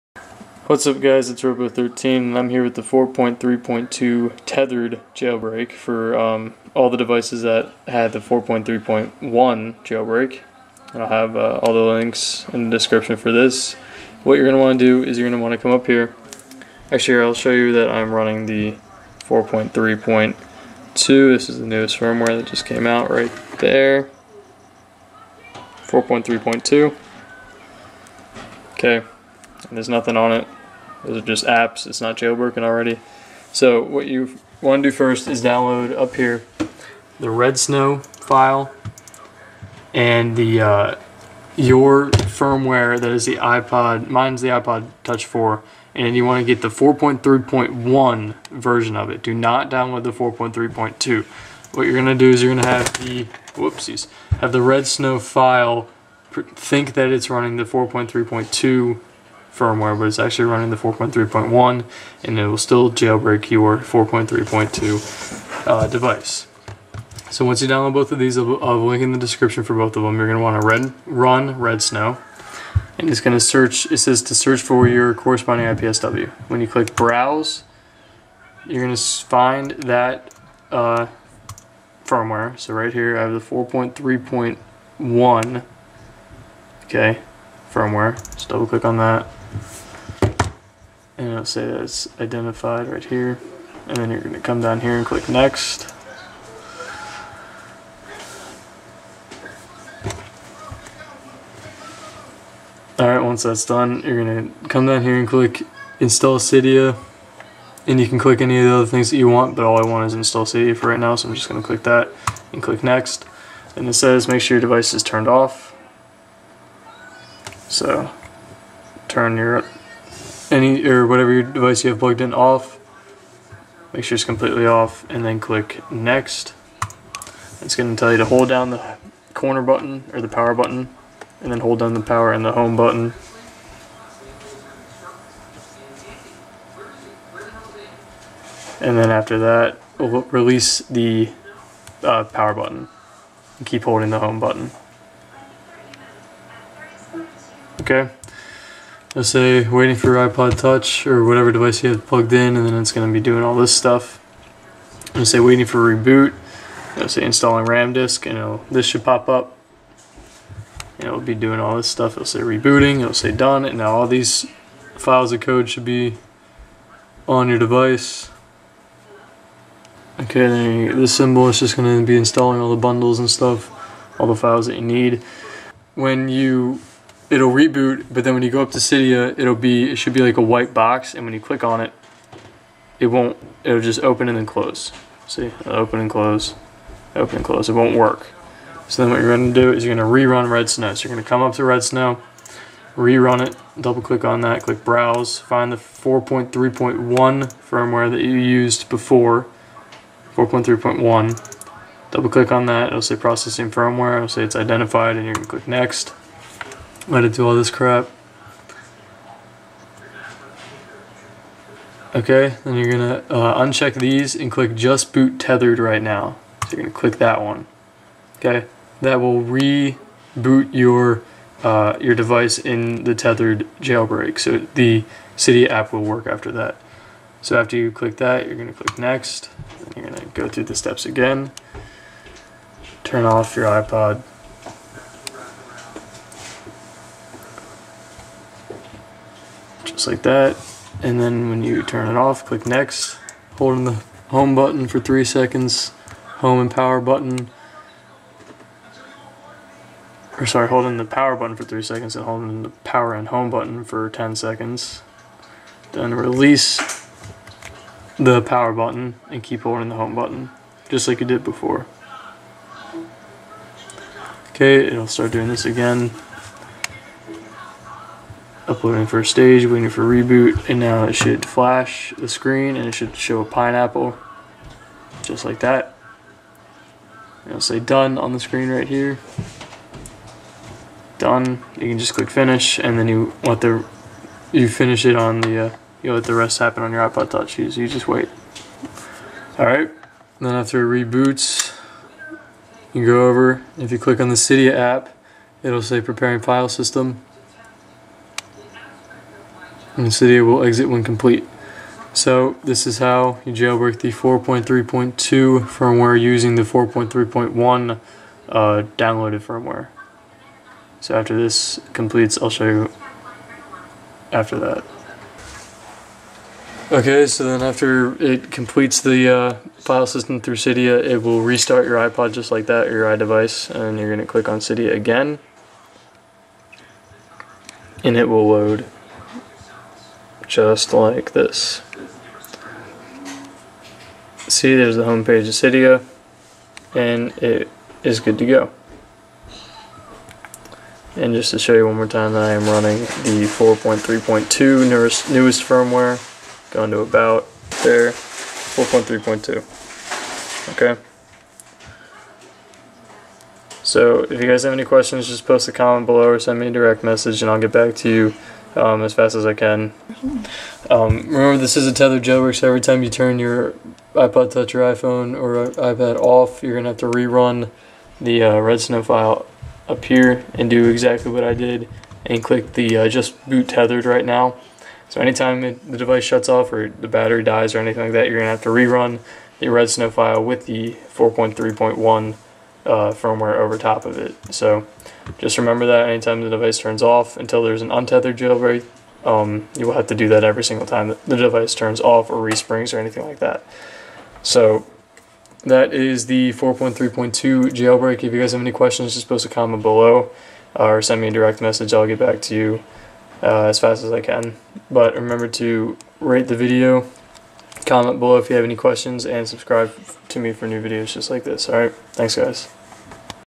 How to Jailbreak 4.3.2 Firmware Tethered With RedSn0w 2011!!!
How to jailbreak 4.3.2 firmware on tethered jailbreak.

Devices supported:
Iphone 4 (AT&T)
Iphone 3GS (old and new bootrom)
ipod touch 4th generation
Ipod touch 3rd generation
Ipad 1